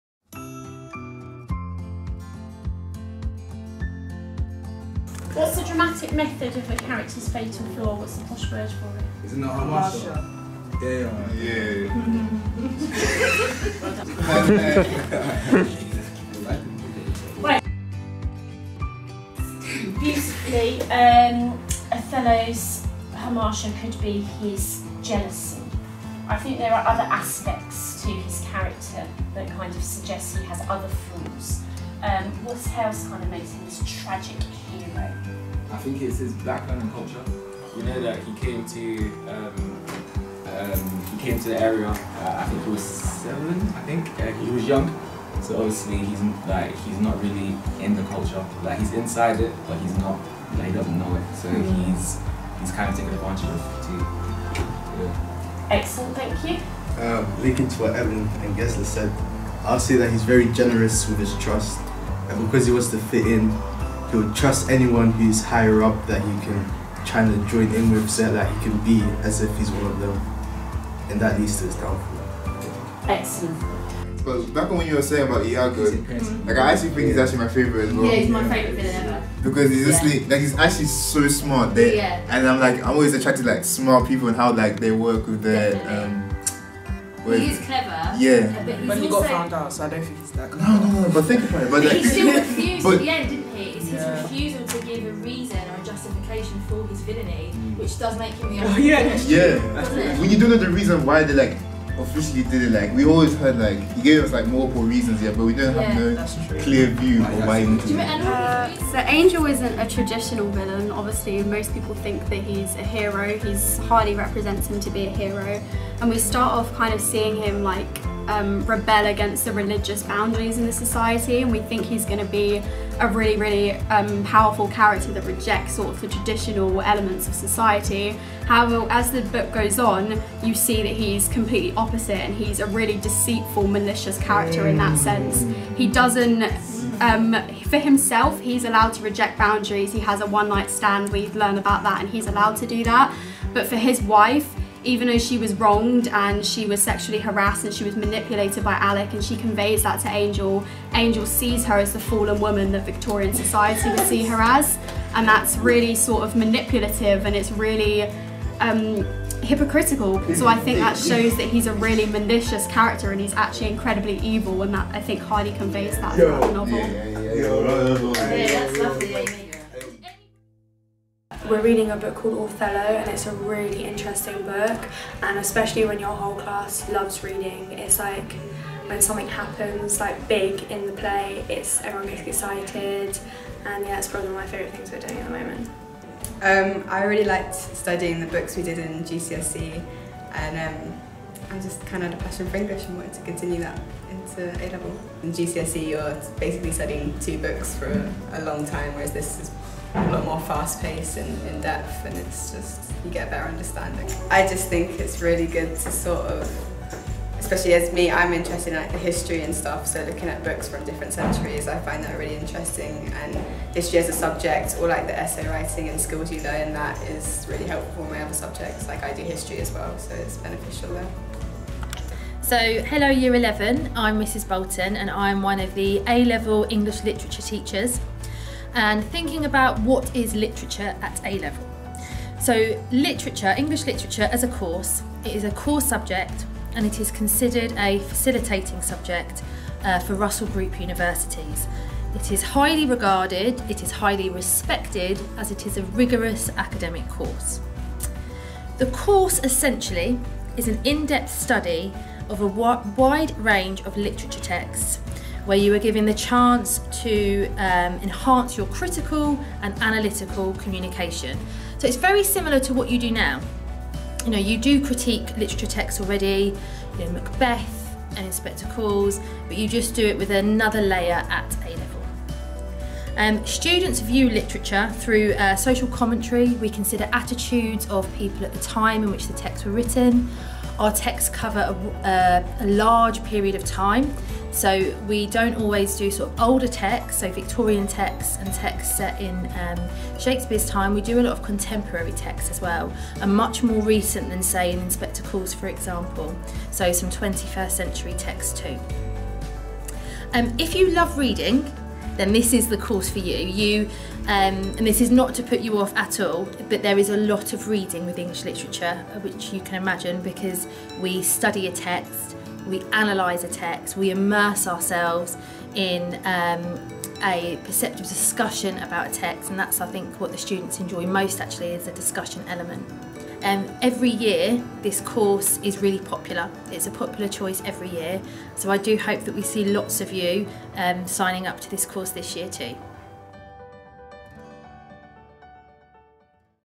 A-Level English Literature
Watch this video to find out about studying A-Level English Literature at St Paul's Catholic School, Milton Keynes.